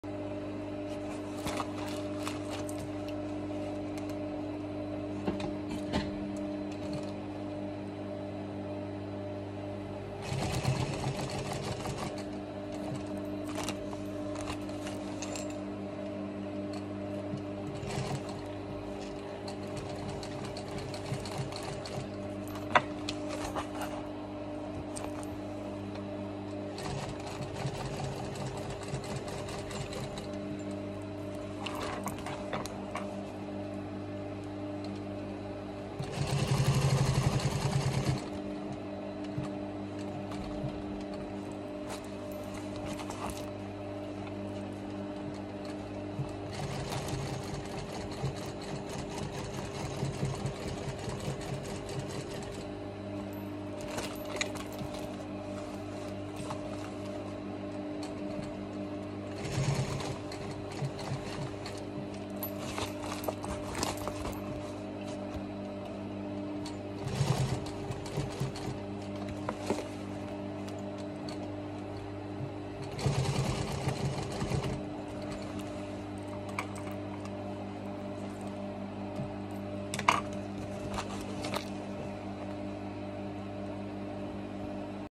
Kbags Unique handmade handbag 👜The process of sewing a design on a bag.
Today I’m making a new handbag. It is a bag with 3 pink hearts in the middle.

It is quite a challenge for me every time I make a new bag.